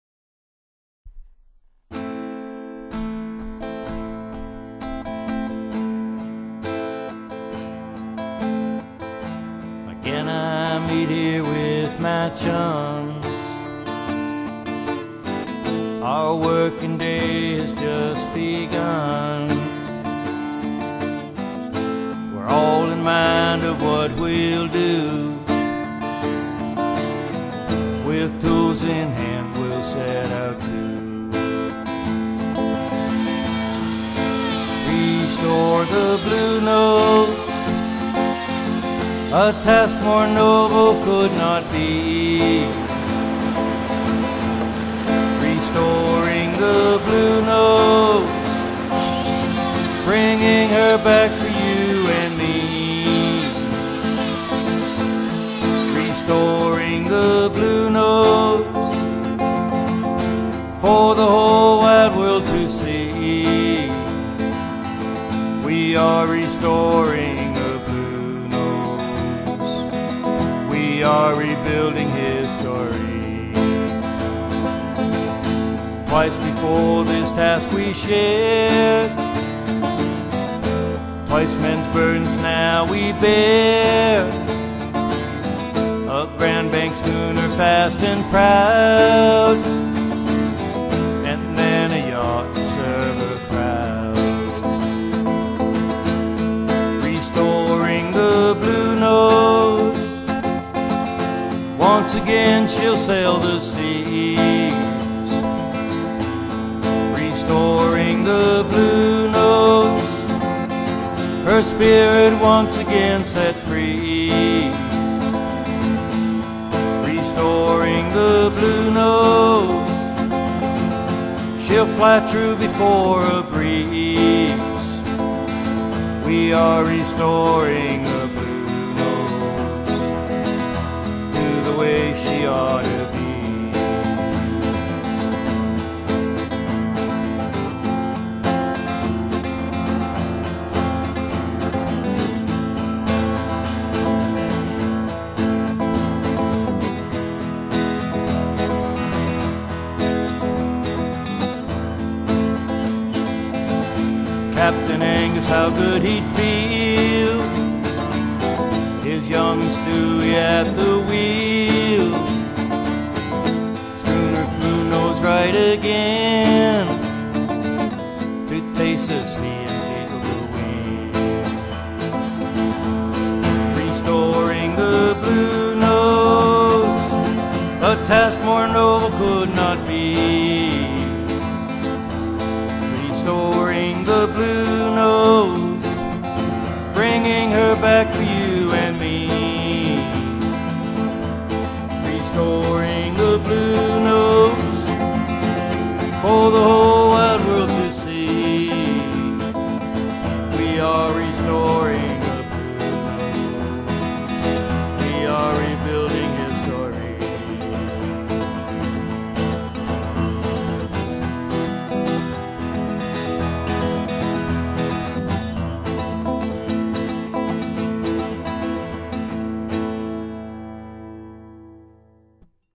Inside Bluenose II - August 2011
Inside the restoration of Bluenose II, this time around we find that the interior is really taking shape, while planking on the hull is moving along in fine form.

or, to order photos from official project photographer Mark Doucette,